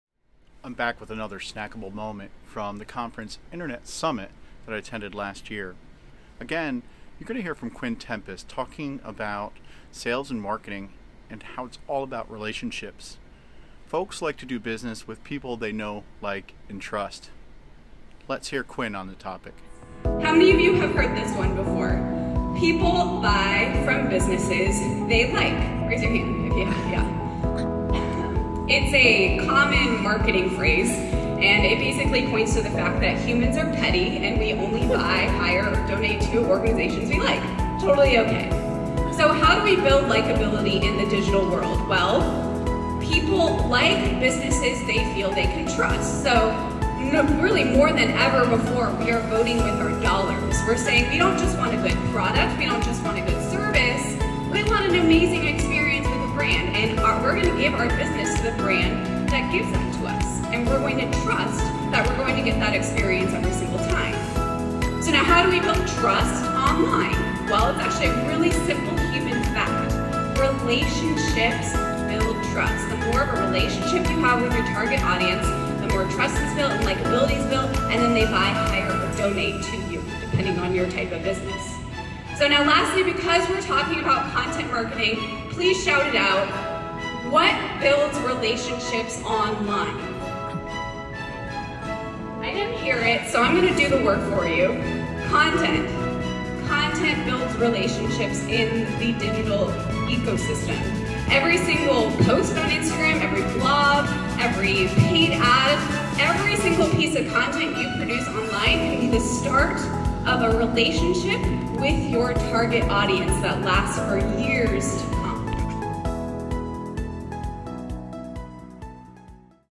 Snackable Moment: Know, Like, Trust
While attending the Internet Summit last year, I was able to listen to a talk from Quinn Tempest in regards to Instagram. But in this snippet, she talks about the importance of building a relationship with your audience and the know, like, and trust factor.

This is not only relevant to Instagram and social, but in sales and marketing in general.